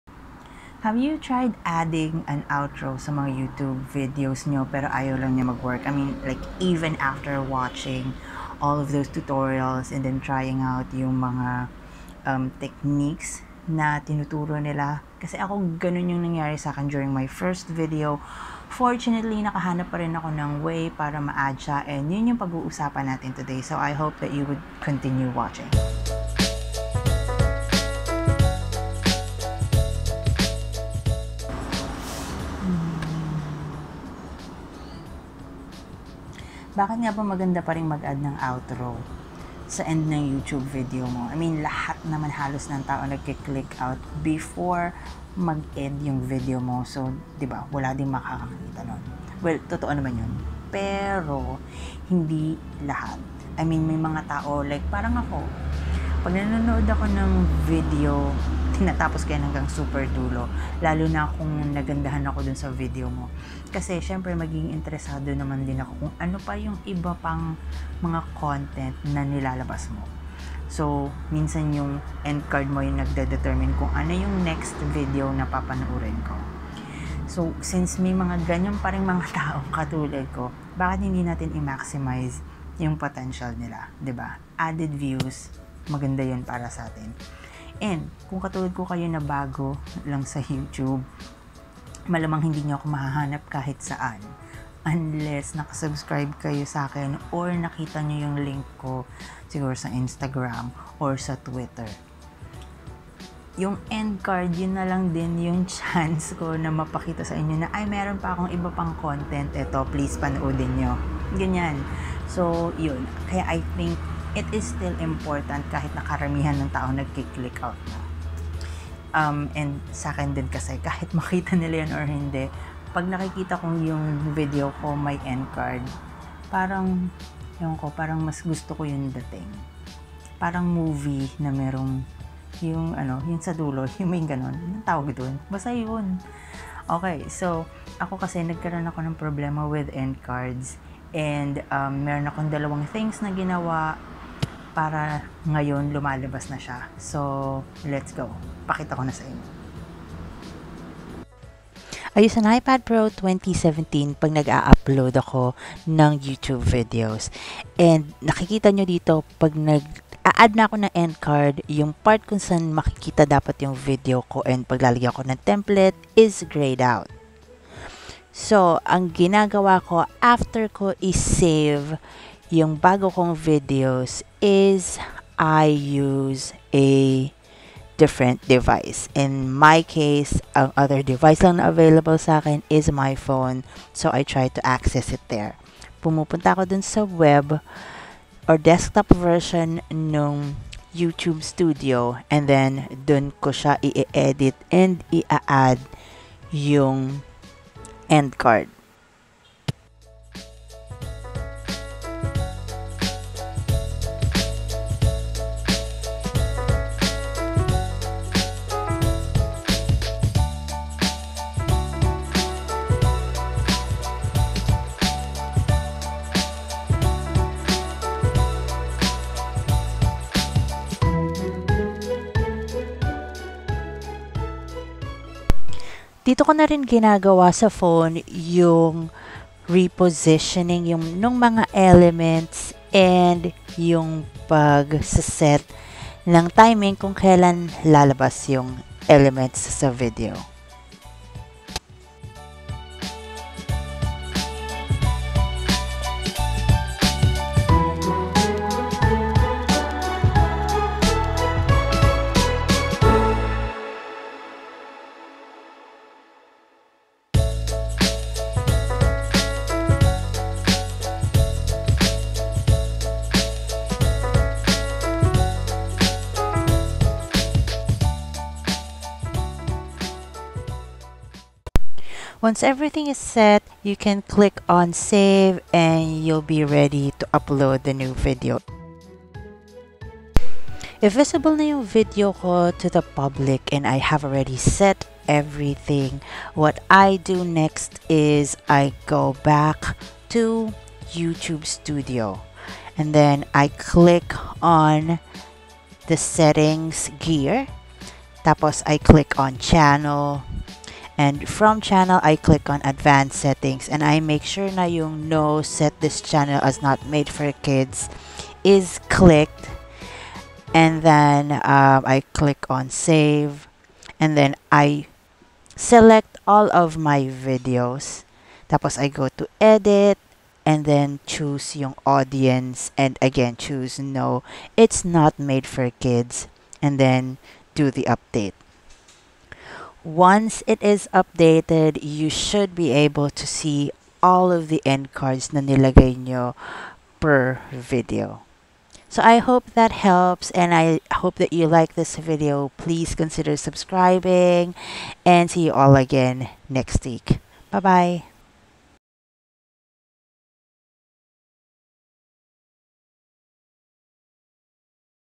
What to do if your end card won’t show up after you’ve set it : Outro tips 2020
Sometimes youtube studio doesn’t show your end cards even if you’ve already set it. This is what I have been experiencing. I upload my videos using an Ipad pro 2017 and when I get to the end card part, my video is on the timeline but not on the view pane. I can add templates but it still won’t show after I’ve completely uploaded. It might be due to the device I’m using so what I do is I use my android phone to set up the outro/ Endcard.

If that still does not work, I check my settings. Even if you have already set that on the specific video, we still have to make sure that out channel settings is set to Not for kids, unless it really is. If your content is for kids, sorry, I don’t know if there’s a way around this.

Stay safe!